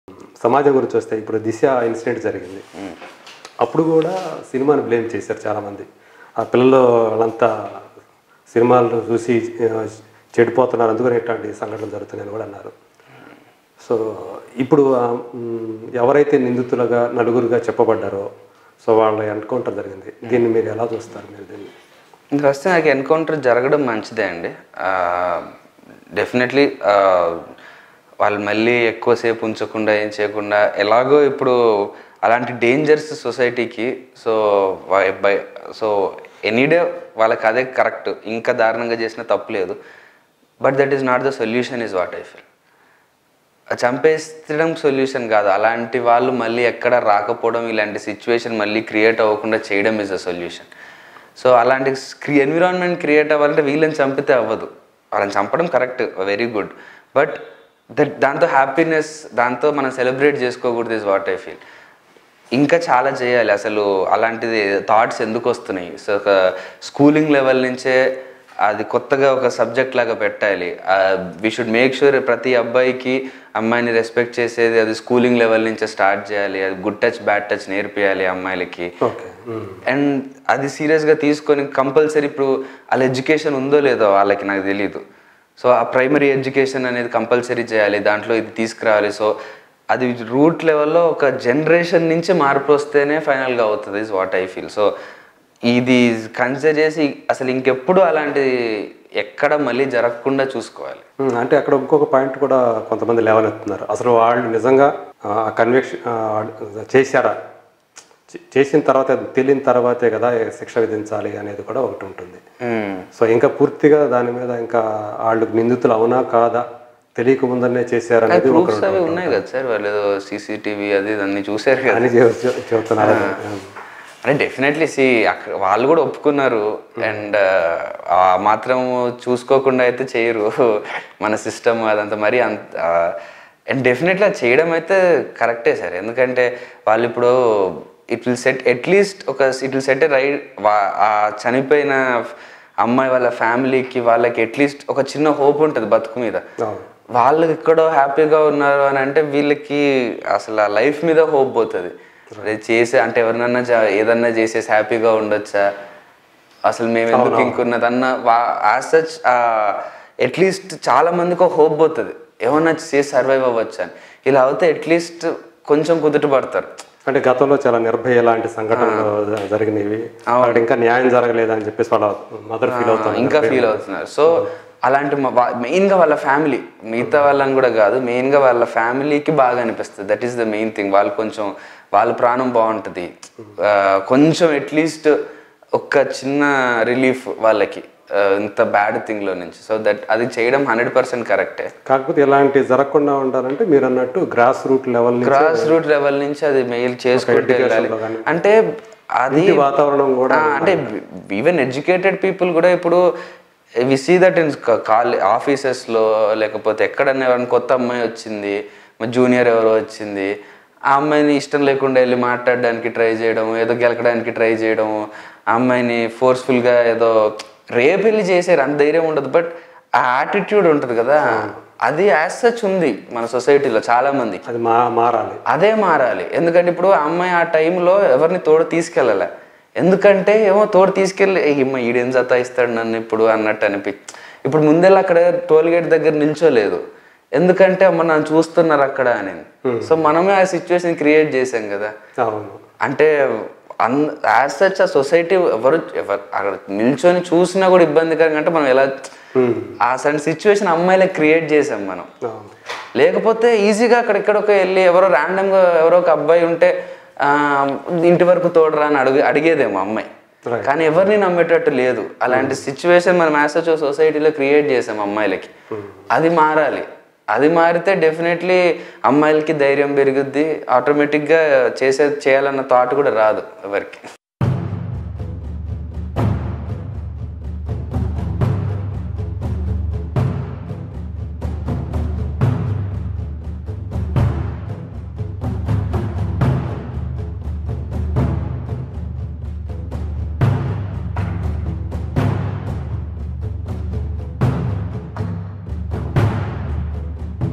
Karthikeya About Disha Case | ఆఫీస్ లో కొత్త అమ్మాయి వస్తే కెలకడం మానాలి | TeluguOne
Hero Karthikeya Latest Interview about Disha Case.